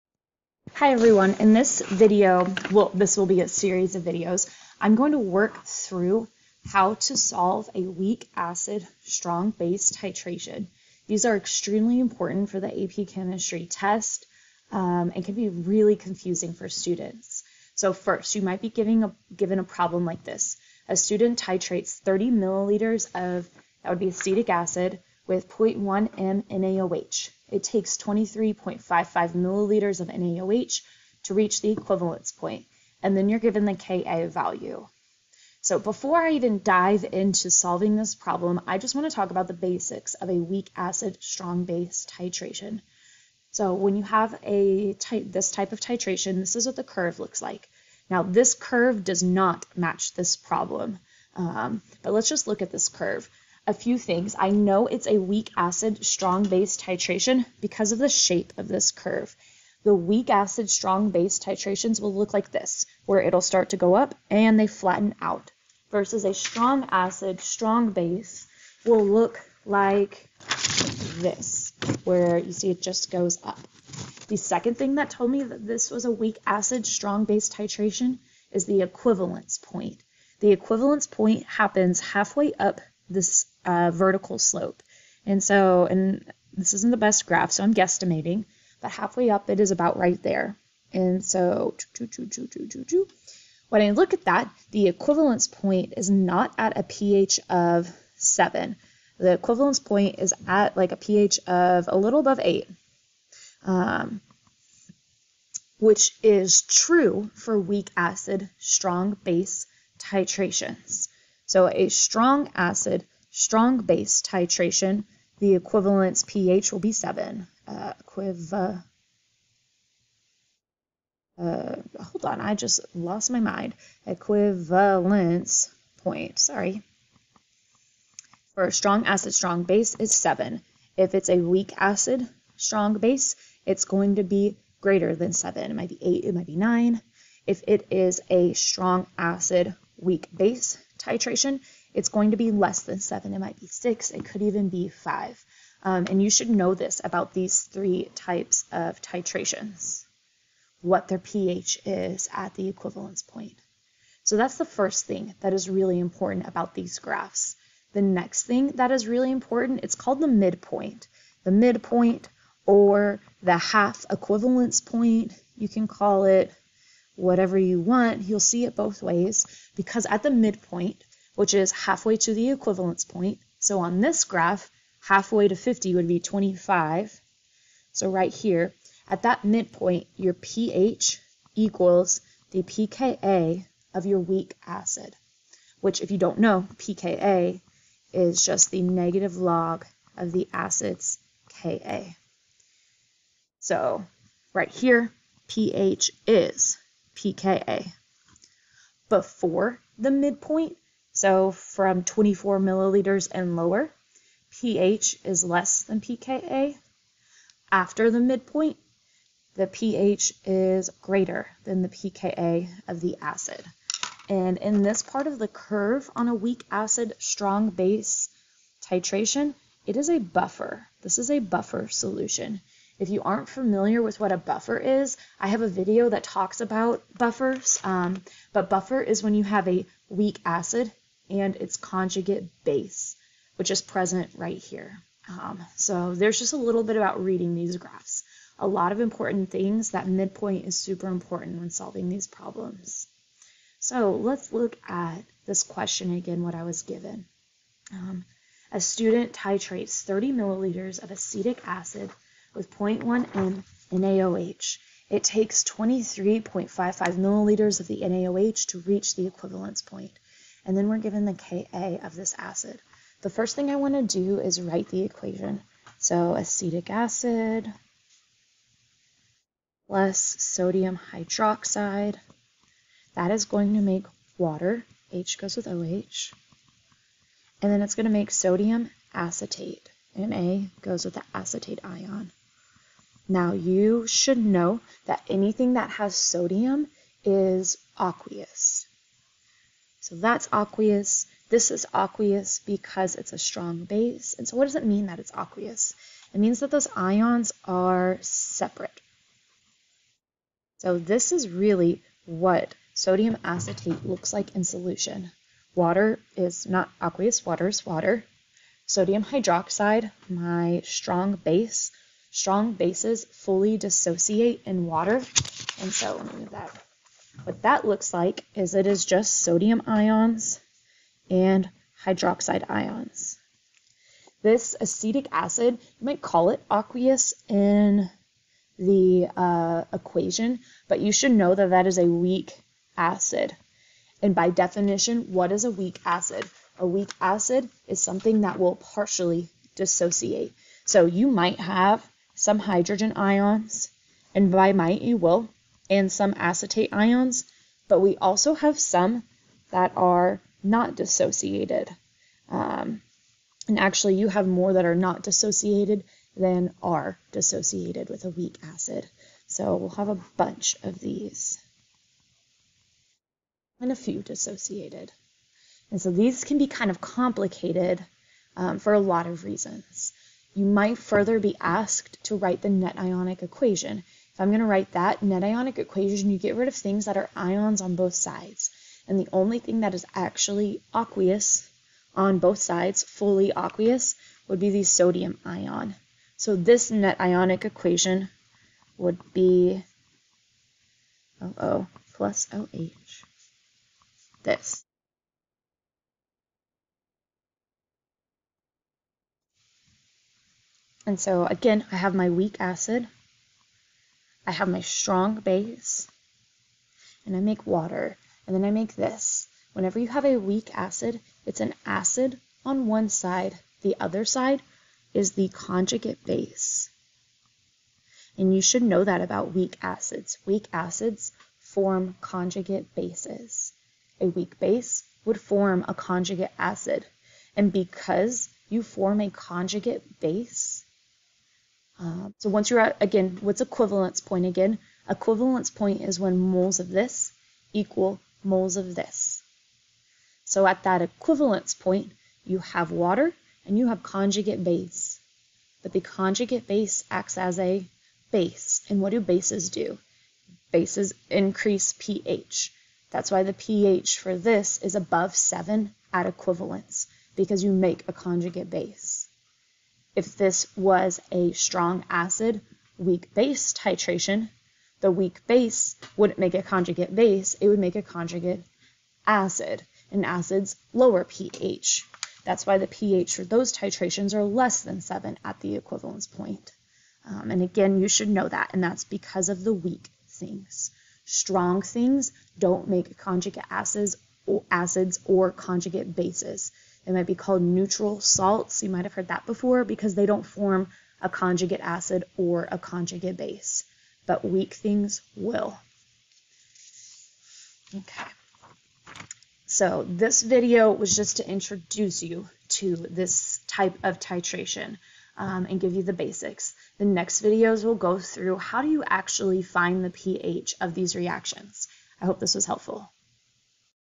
Introduction to Weak Acid/Strong Base Titrations
This video introduces weak acid and strong base titrations along with how to read these titration curves